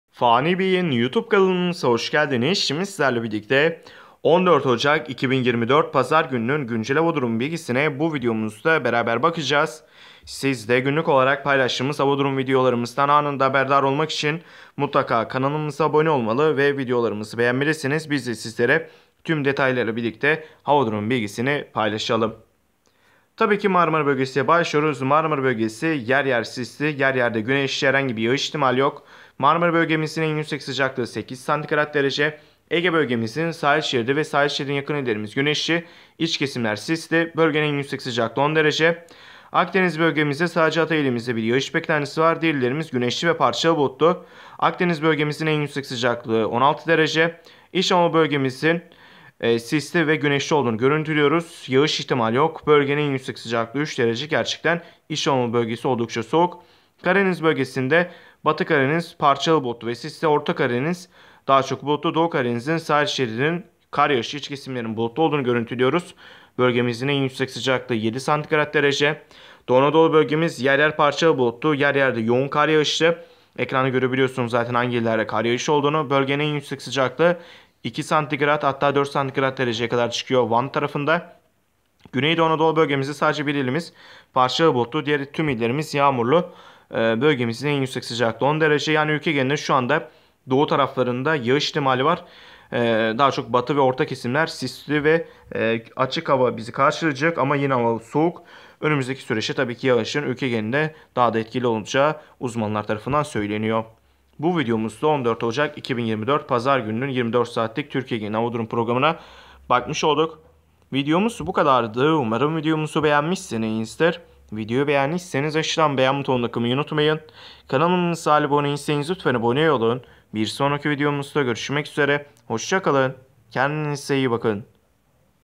14 Ocak 2024 Hava Durumu (Tüm İllerin Hava Durumu)
Merhabalar fani beyin youtube kanalımıza hoşgelginiz bu videomuzda sizlere 14 ocak 2024 pazar gününün Türkiye geneli hava durumunu aktarıyoruz.İyi Seyirler Dilerim.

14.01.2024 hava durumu
14 ocak hava durumu